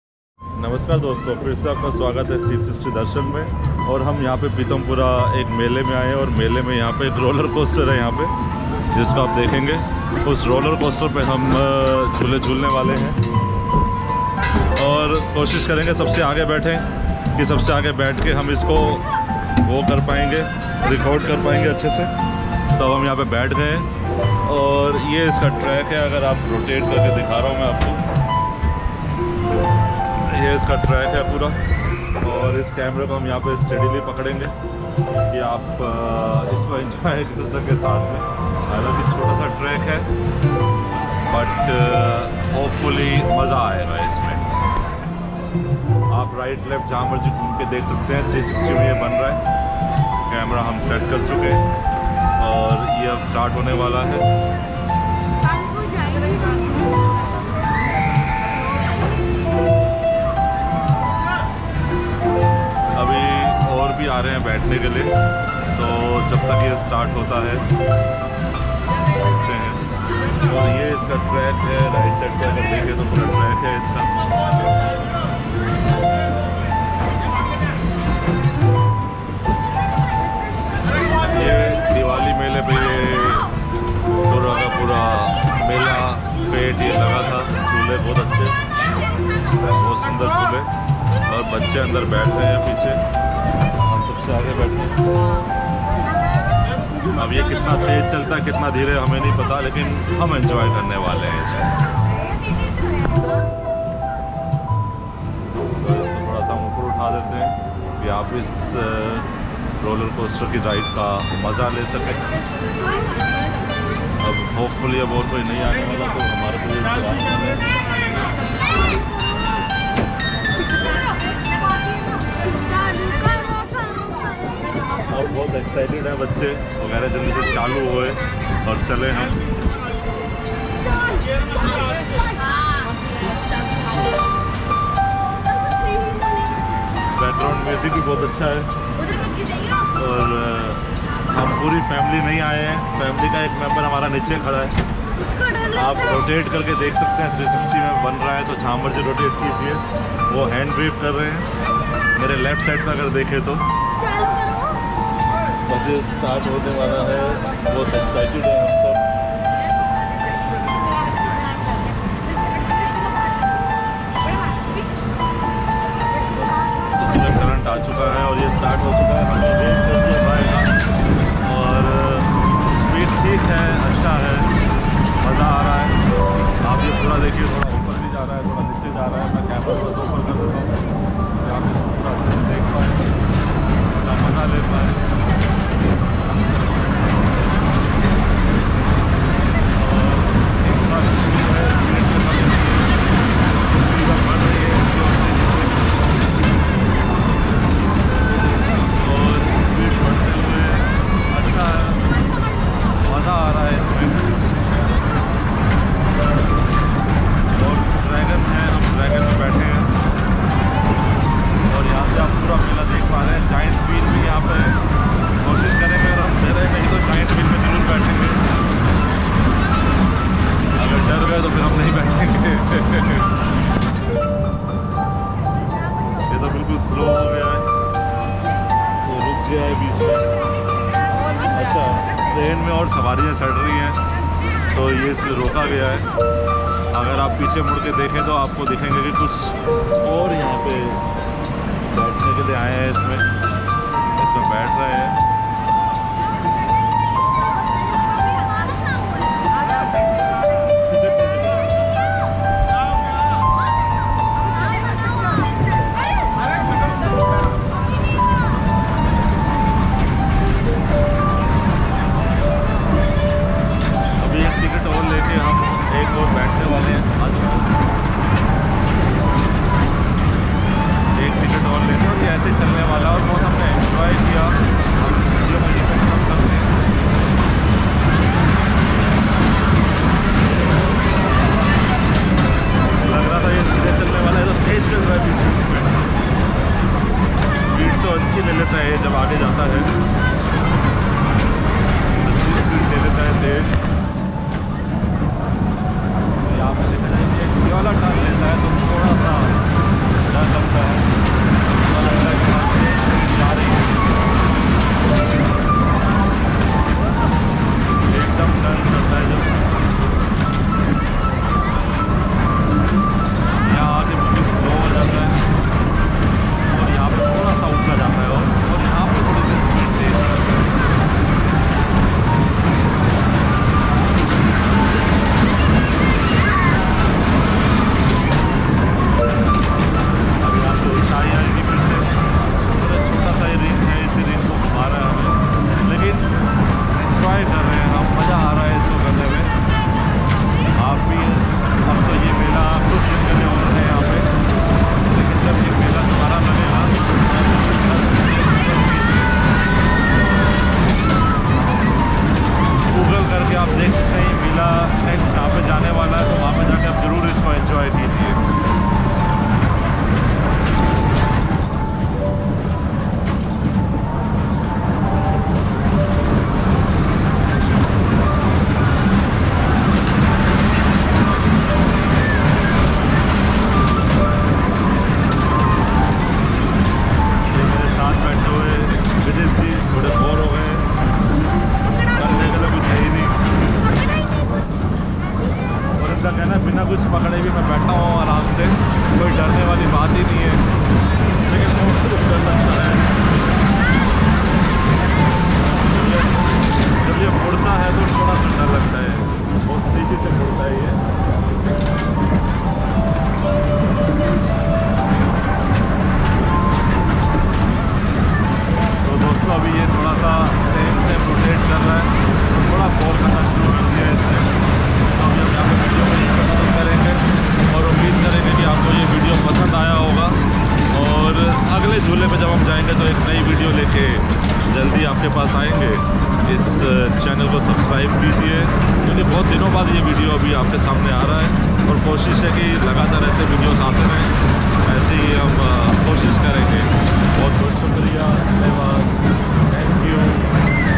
2. 360° Diwali Mela Apeejay School (Pitampura) 360° VirtualTour VO | GoPro MAX 360° 5.7K
2. 360° Diwali Mela Apeejay School (Pitampura) 360° VirtualTour VO | GoPro MAX 360° 5.7K